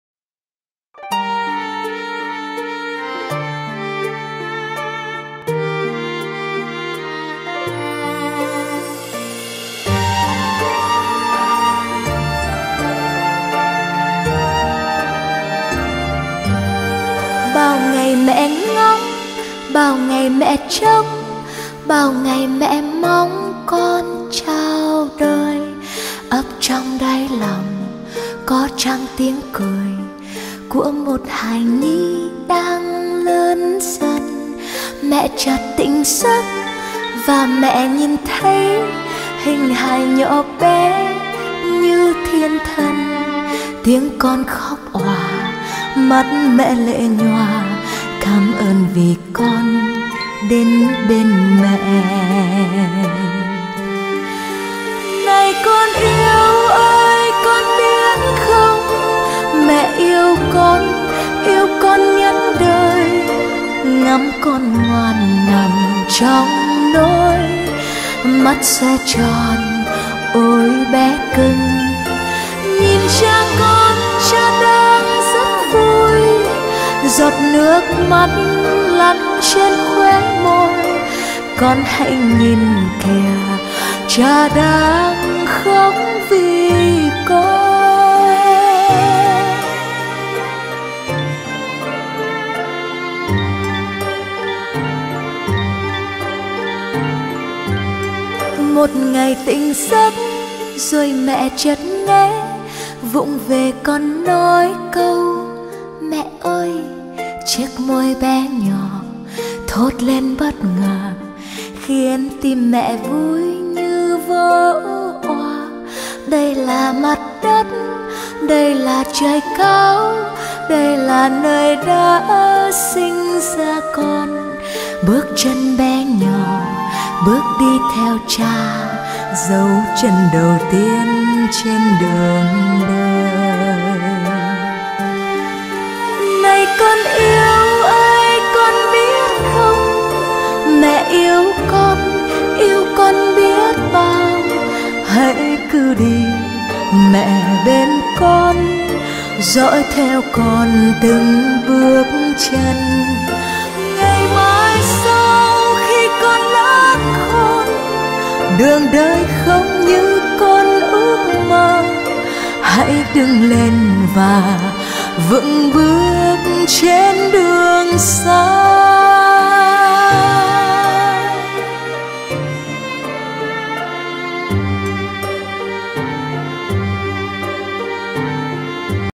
This video is about our dear Beo - standard poodle puppy.
Beo is a son of Luna and Joey. His were born in Hanoi on 24 April 2014. He stays with us the longest before leave to his new home. This video clip is a collection of photo we took during his innitial stay with us.